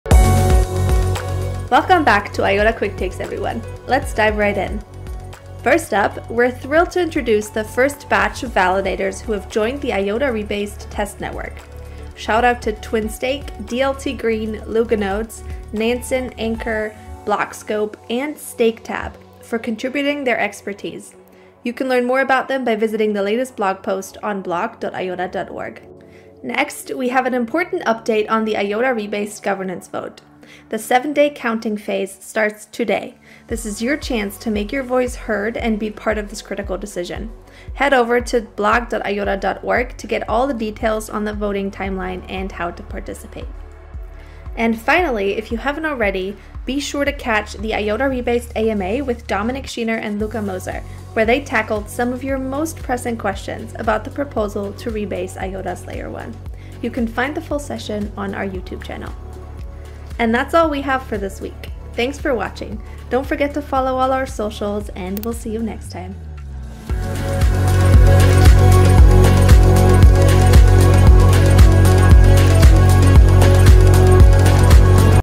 IOTA Quicktakes 09.12.2024: First Validators Join IOTA Rebased Test Network, Vote Counting Begins
[IOTAQuicktakes] Time to recap our most recent news:
🌀 First batch of validators joined the IOTA Rebased test network!
🗳️ IOTA Rebased vote counting phase begins today
🎥 Watch the Rebased AMA with Dominik Schiener and Luca Moser